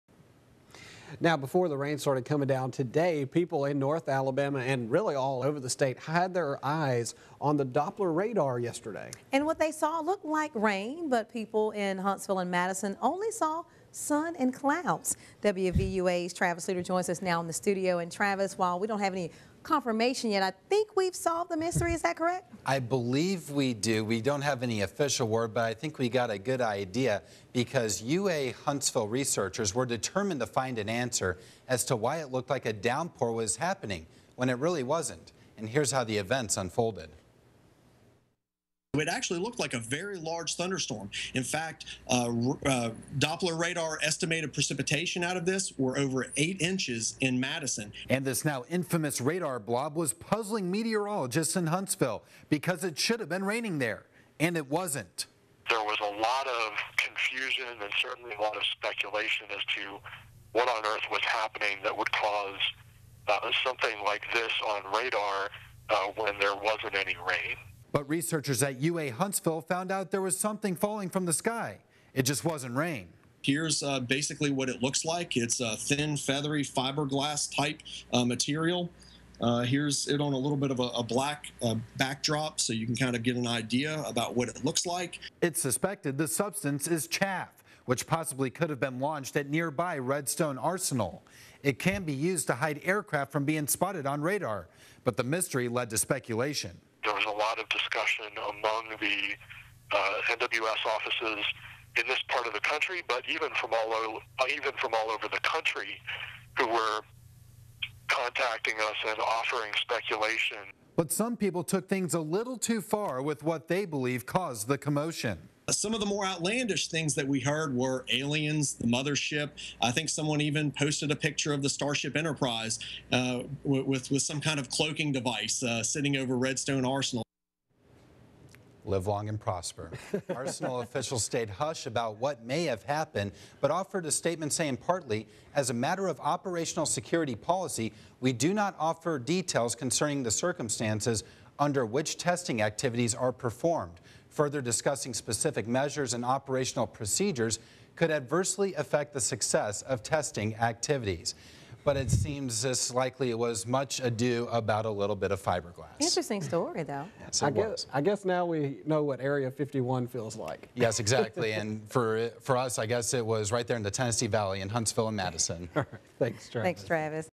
Mystery Radar Blob
Huntsville meteorologists piece clues together to find out what caused a massive "blob" to appear on Doppler Radar.